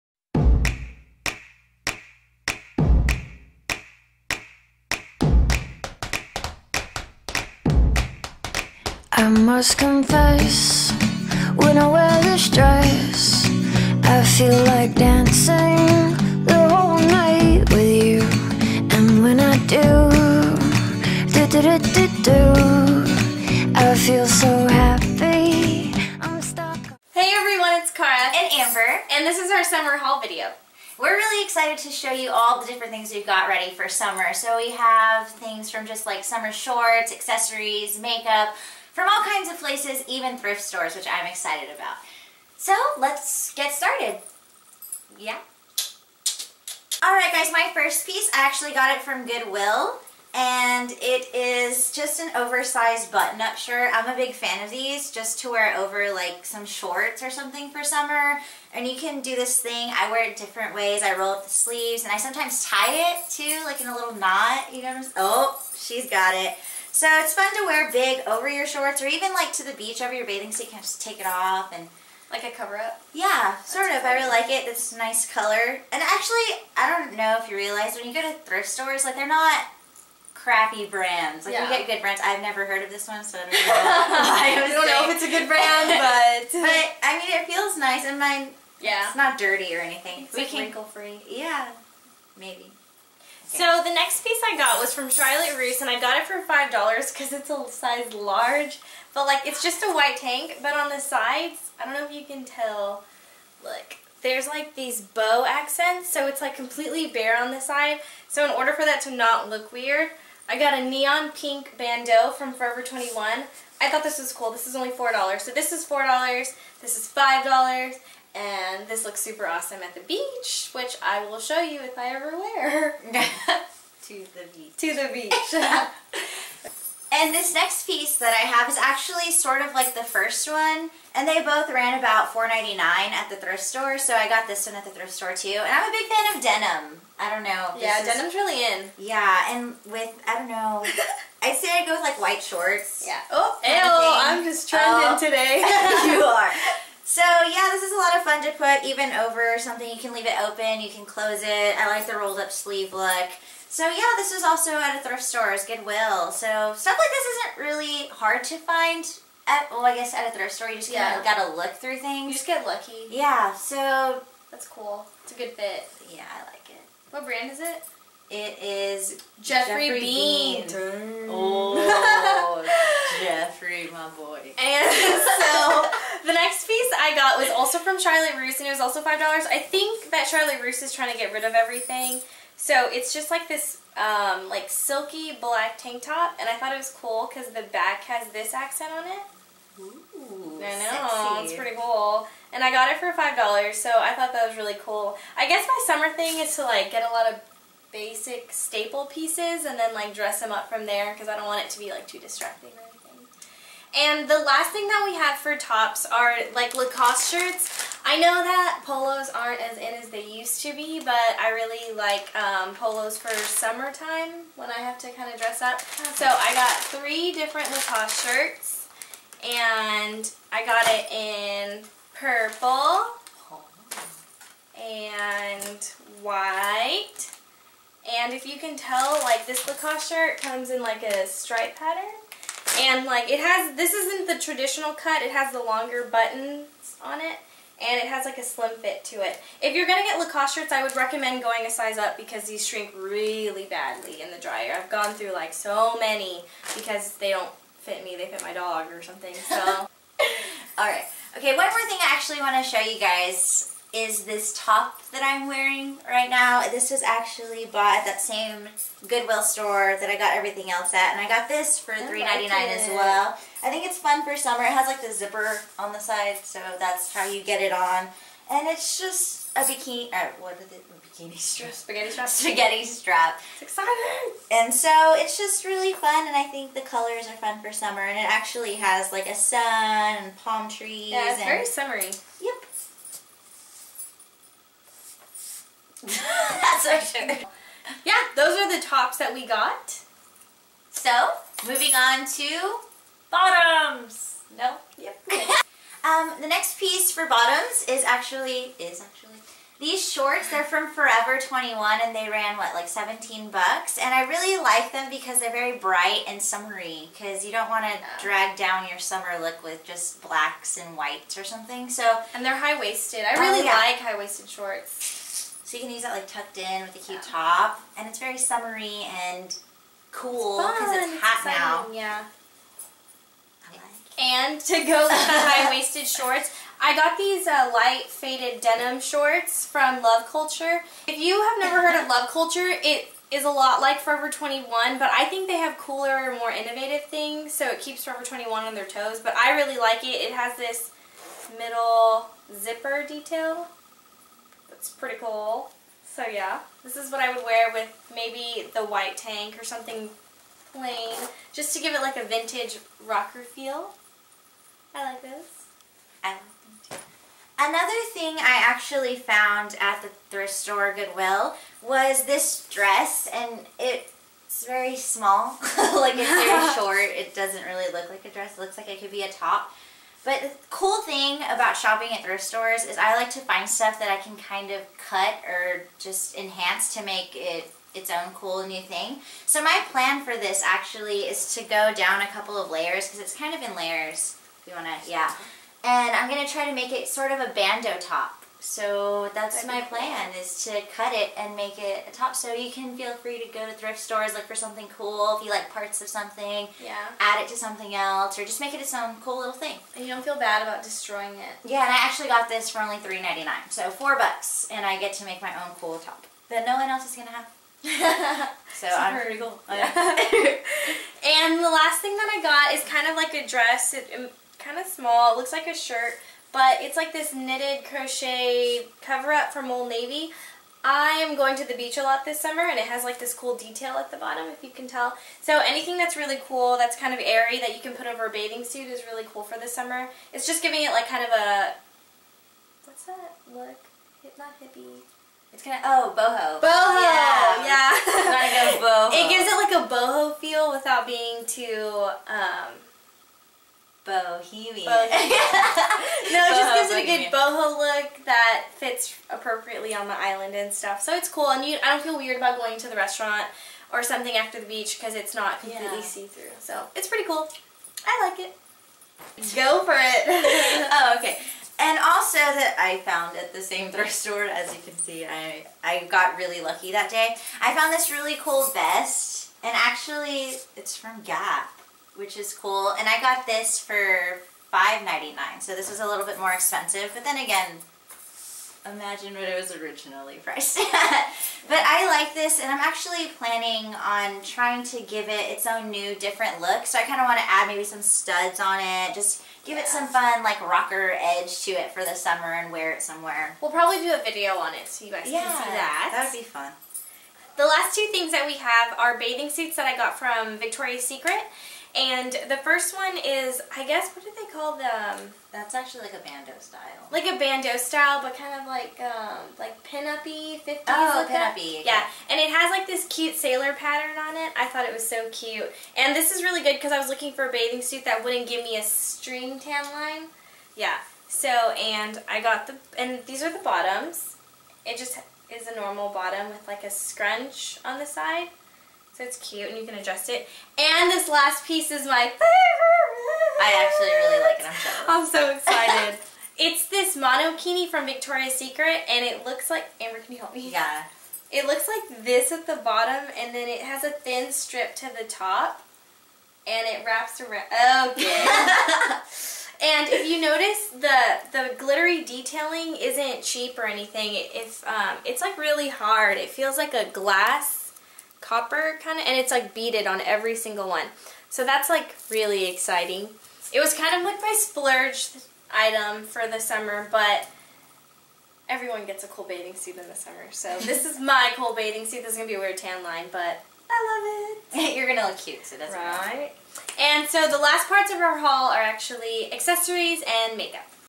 Summer Haul Pt 1: Summer Tops, Bathing Suits, and High Waisted Shorts!
A summer themed haul video with clothes from Charlotte Russe, Lacoste, Victoria's Secret, Love Culture, Forever 21, and THE GOODWILL THRIFT STORE! :P